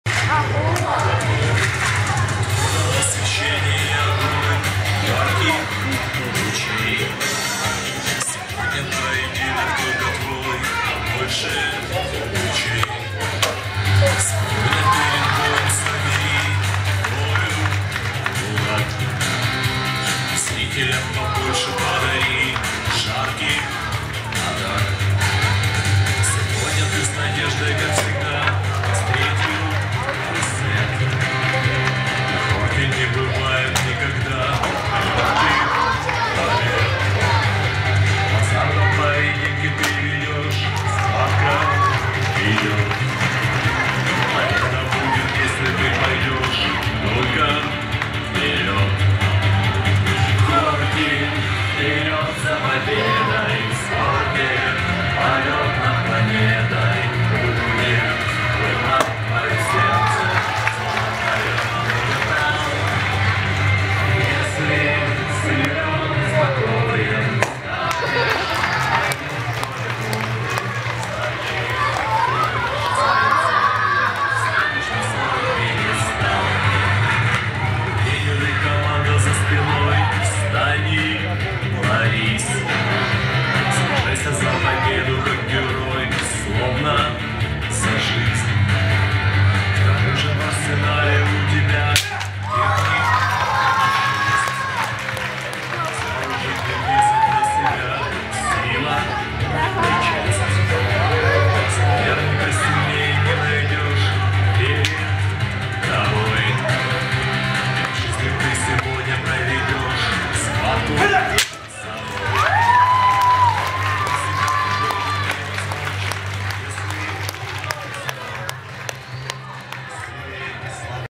Чемпіонат Київської інженерної гімназії з хортингу
Відкритий Чемпіонат Київської інженерної гімназії з хортингу серед дітей шкільного віку у розділі змагань «Форма», присвячений Дню Захисника Вітчизни 23 лютого 2013 року. Голова оргкомітету -- директор гімназії Рудей Дмитро Юрійович. Головний суддя Гречаний Олександр Миколайович. Під керівництвом Президента Київської міської федерації хортингу Петренка Богдана Андрійовича.